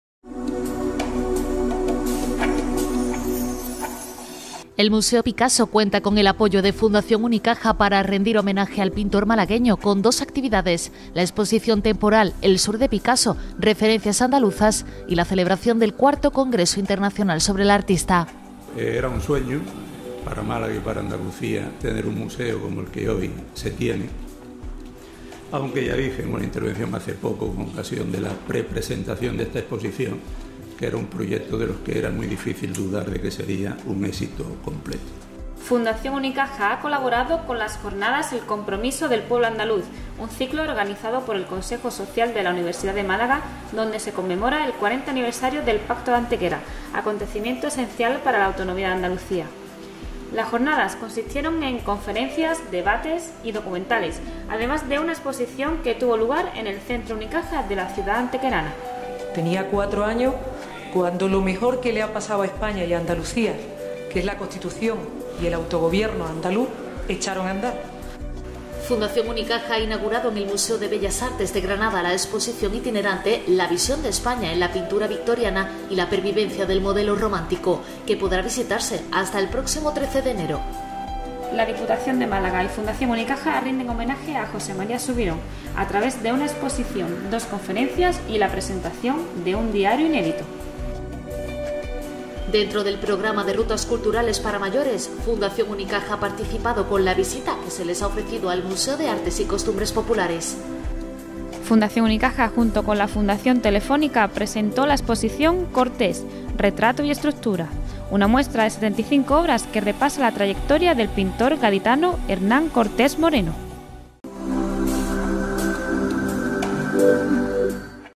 Exposiciones · Fundación Unicaja · Octubre 2018
Algunas exposiciones en las que ha participado Fundación Unicaja en el mes de octubre de 2018. Recuerda que puedes consultar todas las actividades de la Fundación Unicaja